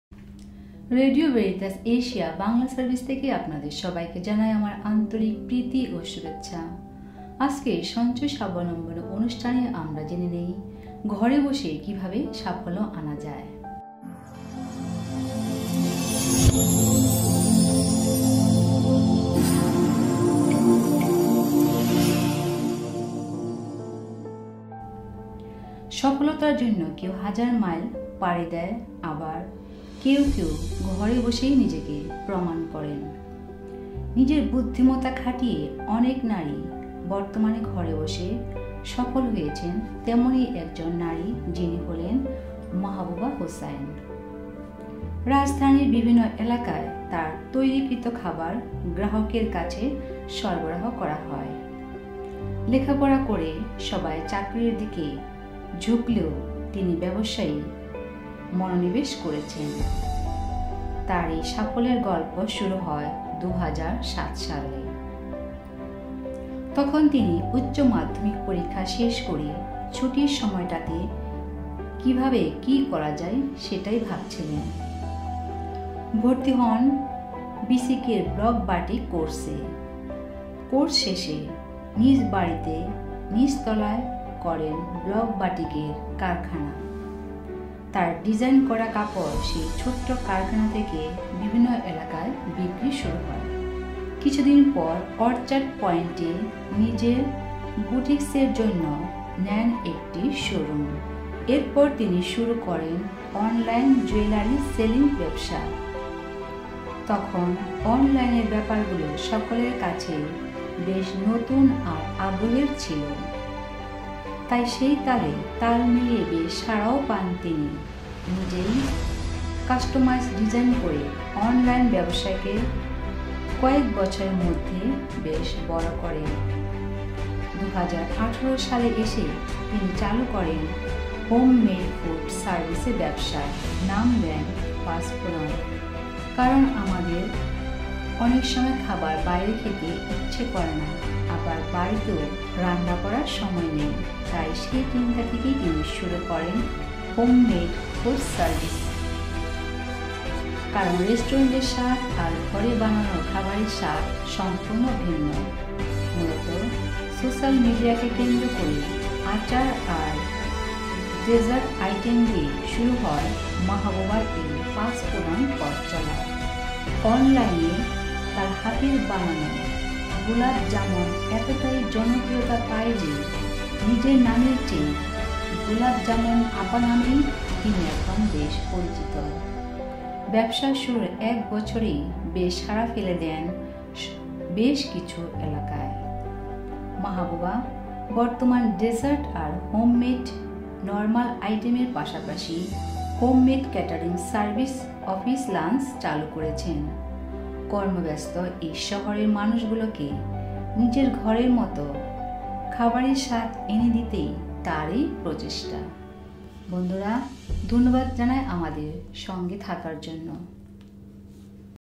সঞ্চয়ে সাবলম্বন
সফলতার জন্য কেউ হাজার মাইল পাড়ি দেয়, আবার কেউ কেউ ঘরে বসেই নিজেকে প্রমাণ করেন। নিজের বুদ্ধিমত্তা খাটিয়ে অনেক নারীই বর্তমানে ঘরে বসে সফল হয়েছেন। আজ শুনবো এমনি এক নারীর গল্প ।